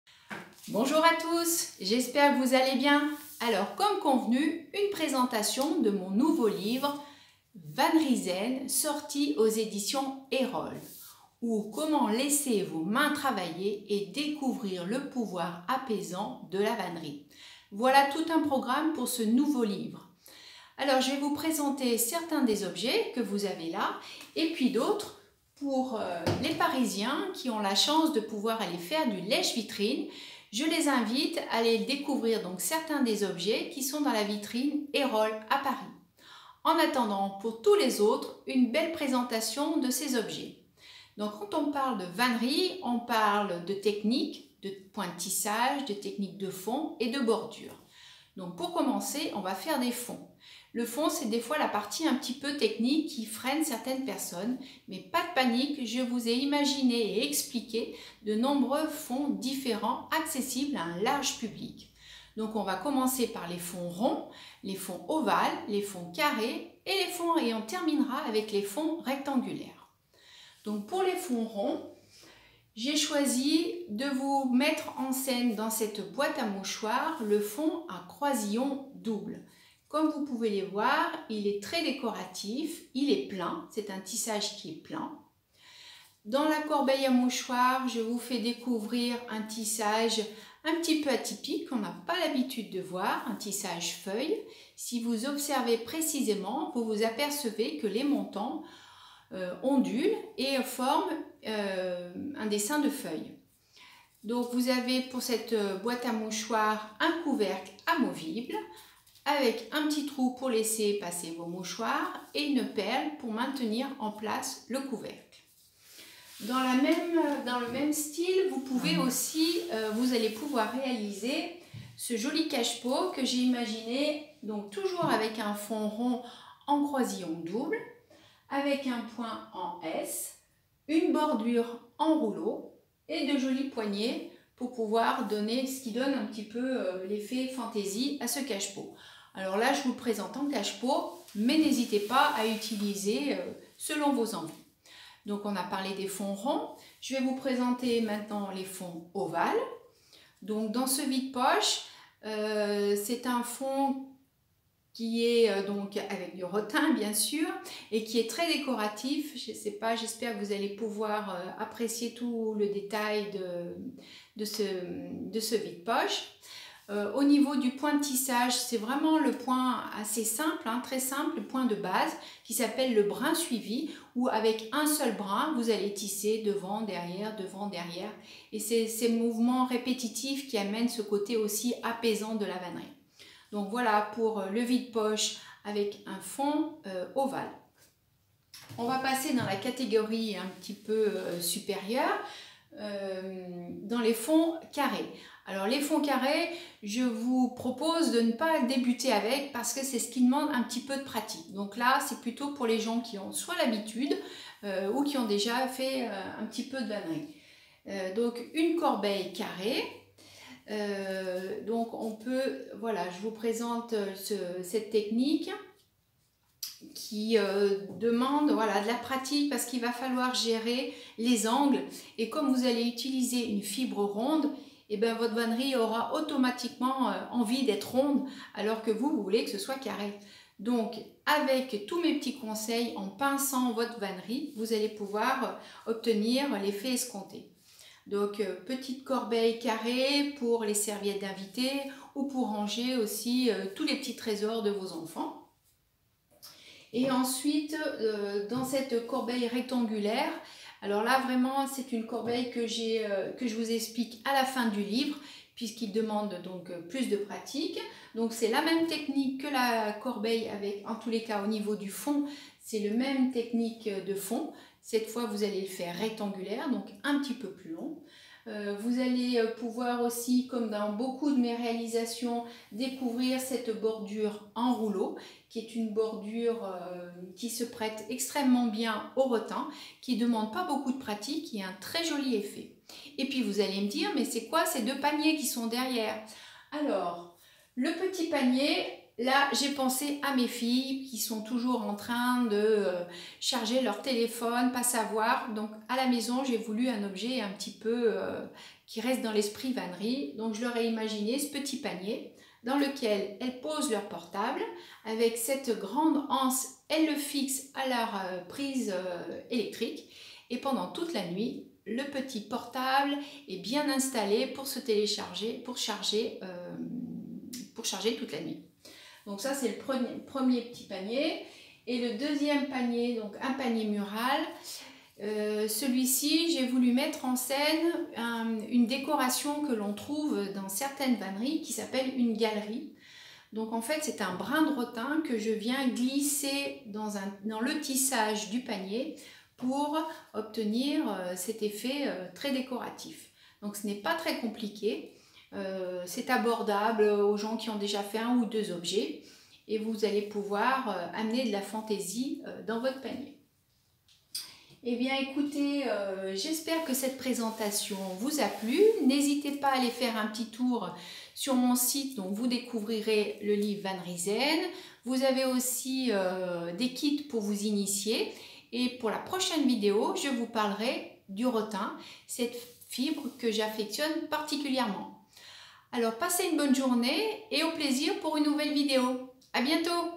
Présentation livre vannerie zen
Laissez travailler vos mains et découvrir le plaisir apaisant de la vannerie.